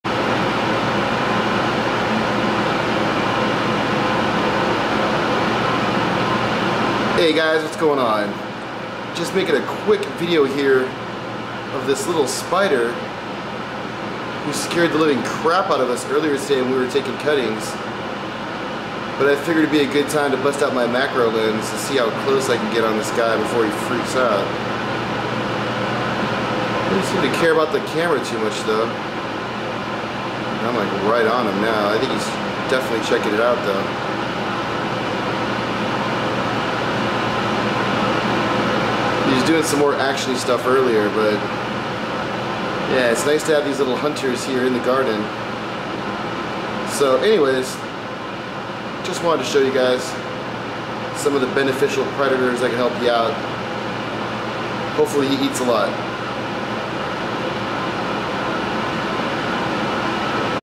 Cannabis Spider Buddy!
Lets see how long he will last!

I will continue to make videos about him until he dies or until I can't find him anymore.